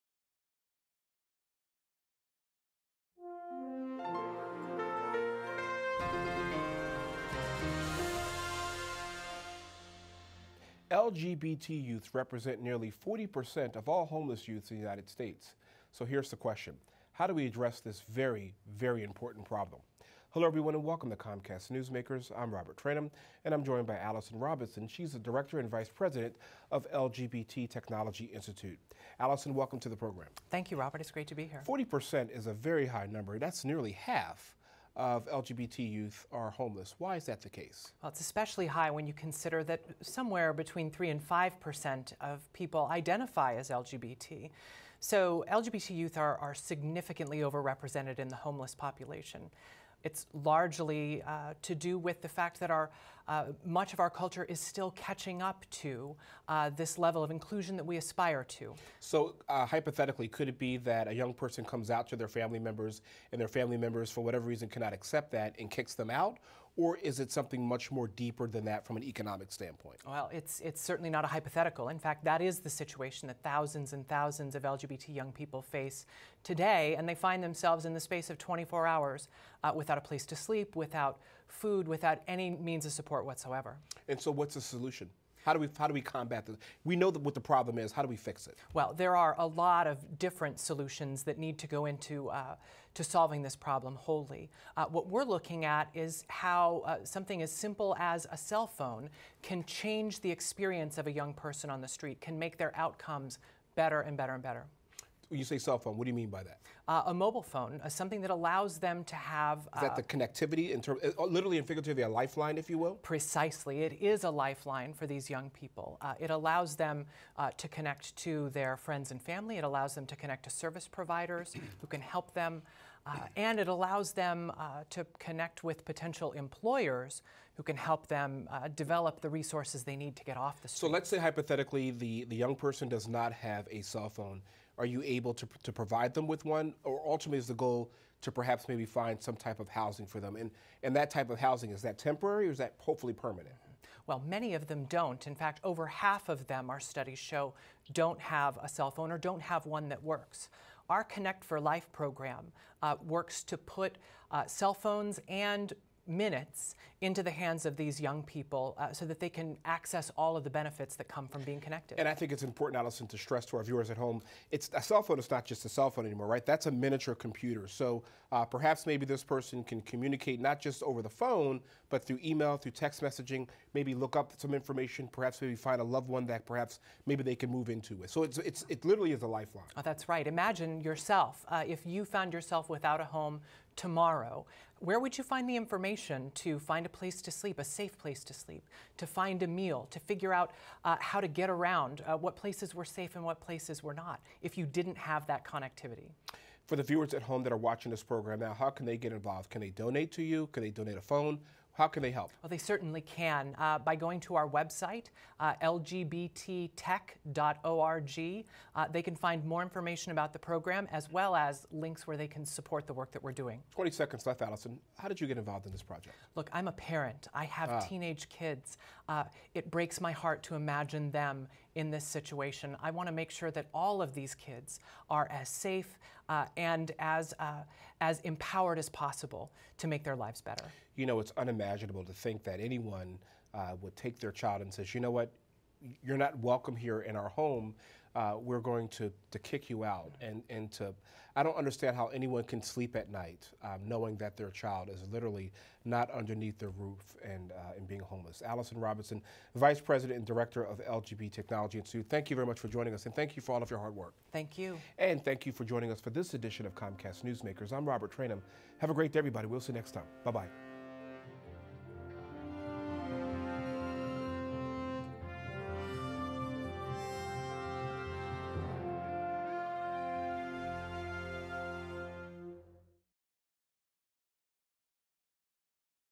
Allyson Robinson on Homeless LGBT Youths
LGBT youths represent nearly 40% of all homeless youths in the United States. Allyson Robinson of LGBT Technology Institute & Partnership addresses this problem. Visit the LGBT Technology Institute & Partnership on the web